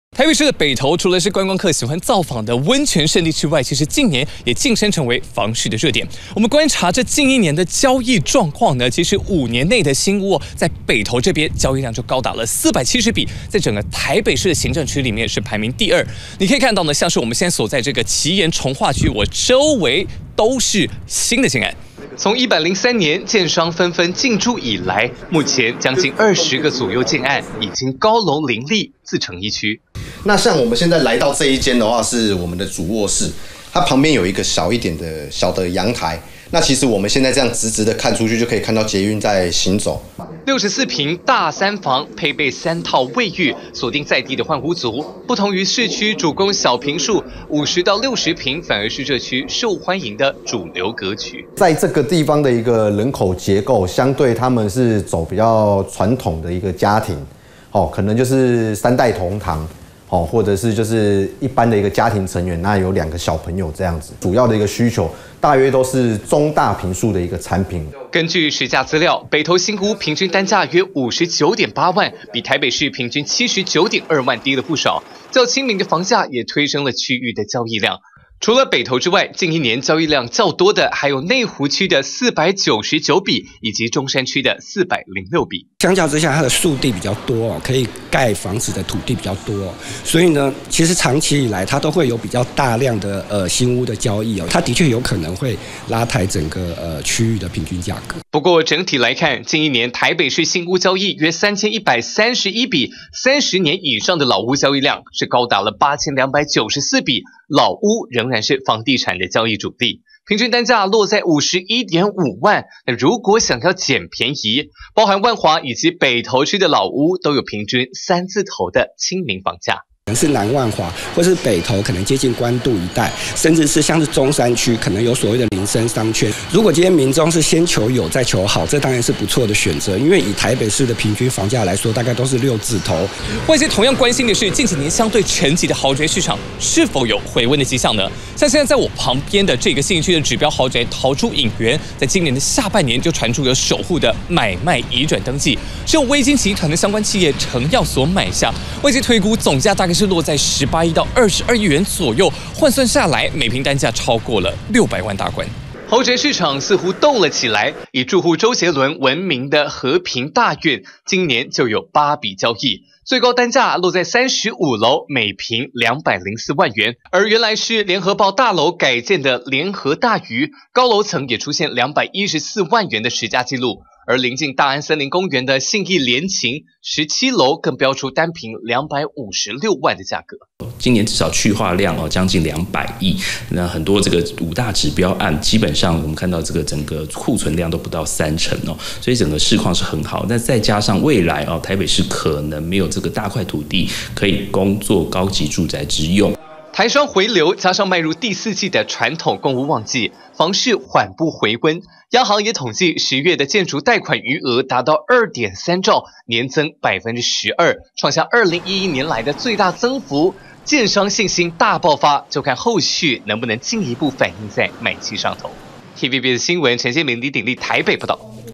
【TVBS新聞｜十點不一樣】《長虹天蔚》北市新屋平均單價79萬！ 想省荷包看這區 20191202
您知道台北市的新屋平均單價多少嗎？統計近一年北市新屋交易資料，每坪均價落在79.2萬元，交易量最大的區域是內湖與北投。若是30年以上老屋，均價仍高達51.5萬元，對首購族來說還是一大壓力，如果想要省荷包，專家建議這兩區三字頭倒是可以考慮。

長虹天蔚
台北市門牌
市區稀有高綠覆的重劃區

席次倒數

歡迎預約賞屋